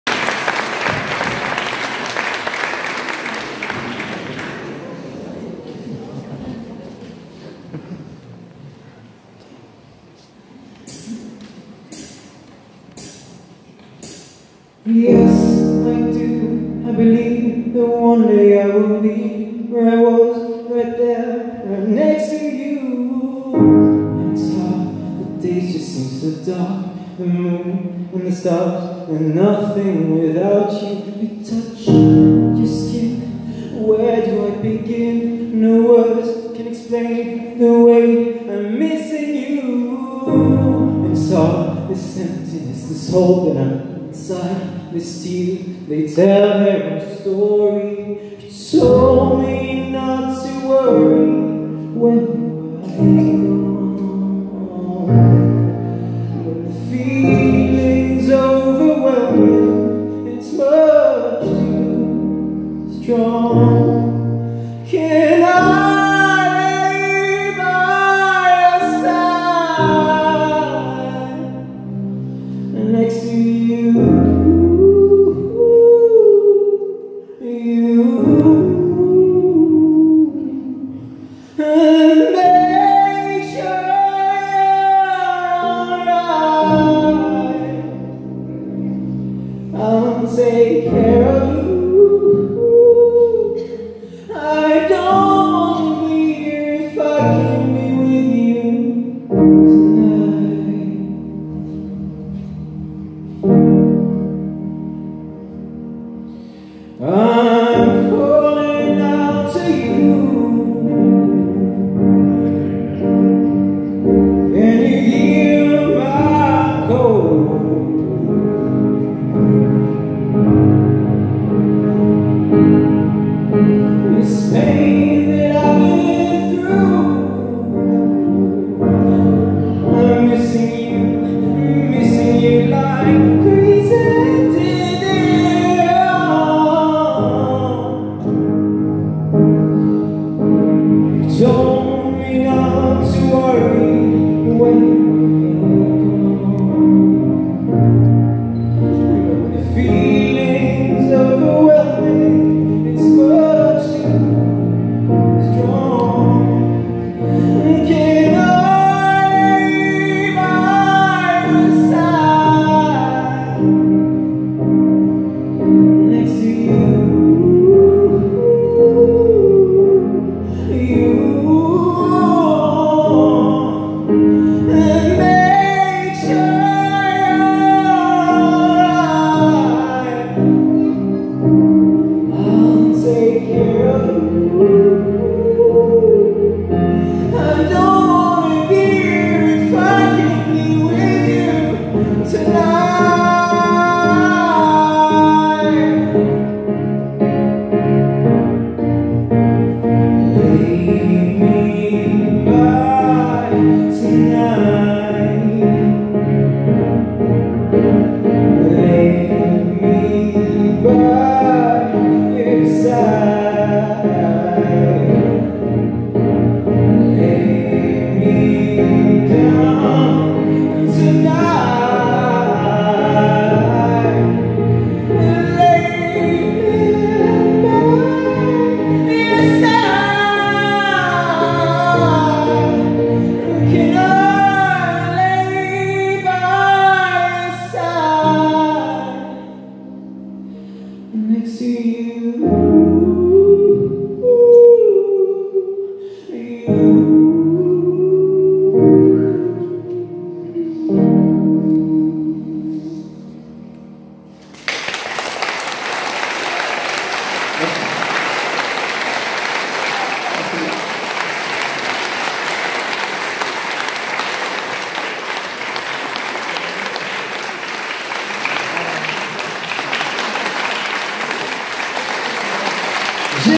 18 novembre 2018
Magnifique  interprétation  au concert de Voices influence gospel  à Brie sous Archiac  le 17 novembre  2018